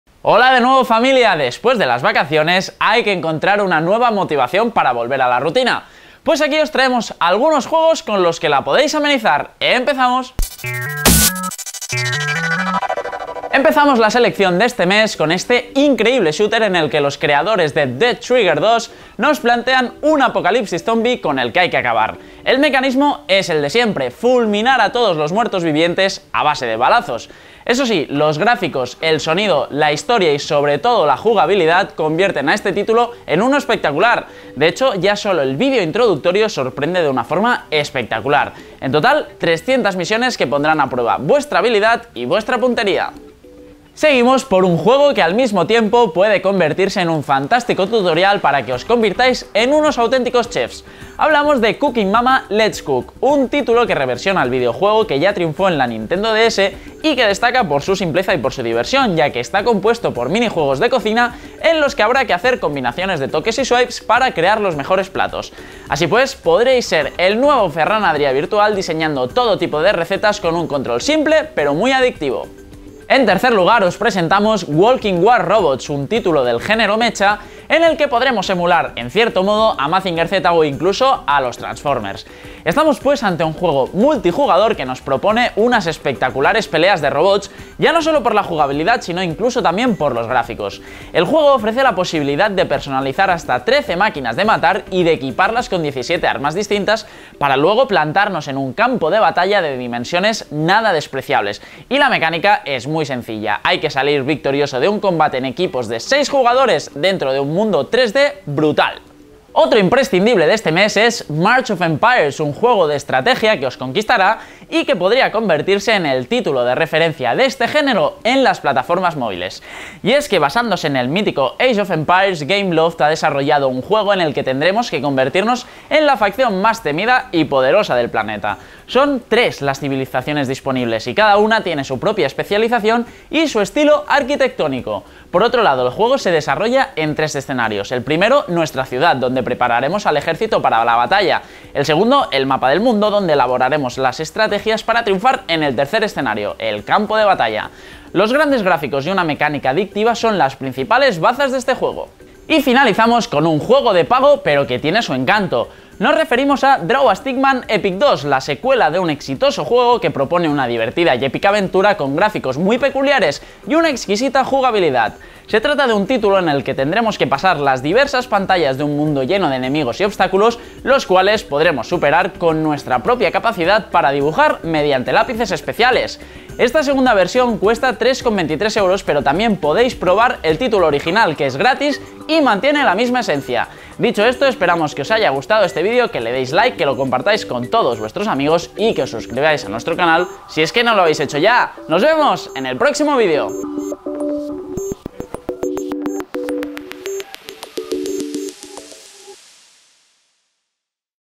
· TOP 5 · Mejores juegos gratis para móvil Android e iOS (Septiembre 2015)
Te mostramos nuestro top5 de los mejores juegos para movil de Septiembrel 2015. ¡Deja el tuyo en los comentarios!

- JUEGOS -